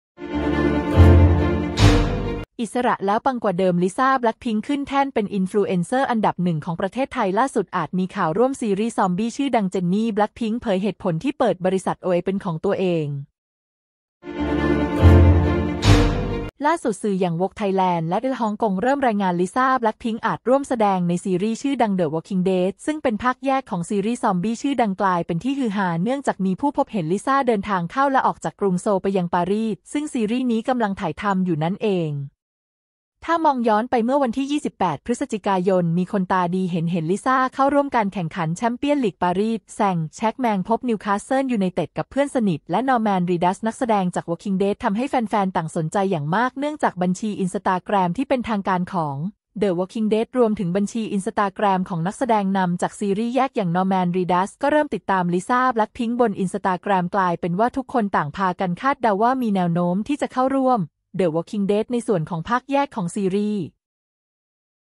อิสระเเล้วปังกว่าเดิม ลิซ่า Blackpink ขึ้นแท่นเป็นอินฟลูเอนเซอร์อันดับ 1 ของประเทศไทย ล่าสุด อาจมีข่
อิสระเเล้วปังกว่าเดิม ลิซ่า Blackpink ขึ้นแท่นเป็นอินฟลูเอนเซอร์อันดับ 1 ของประเทศไทย ล่าสุด อาจมีข่าวร่วมซีรีส์ ซอมบี้ ชื่อดัง เจนนี่ BLACKPINK เผยเหตุผลที่ เปิดบริษัท OA เป็นของตัวเอง